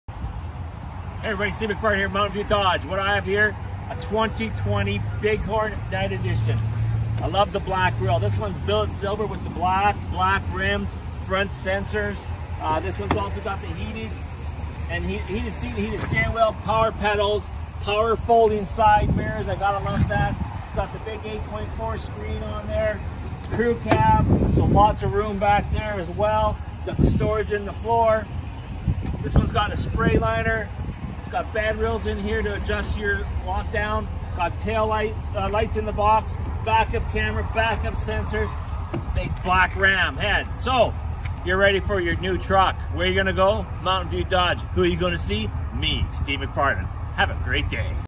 2020 Ram 1500 Big Horn Night Edition | STK # AL076 | Mountain View Dodge
Check out this New 2020 Ram 1500 Big Horn Night Edition Truck Crew Cab at Mountain View Dodge!

STK # AL076

VIN: 1C6SRFFT9LN273763

32580 11 Range Road #43
Olds, AB
T4H 1P6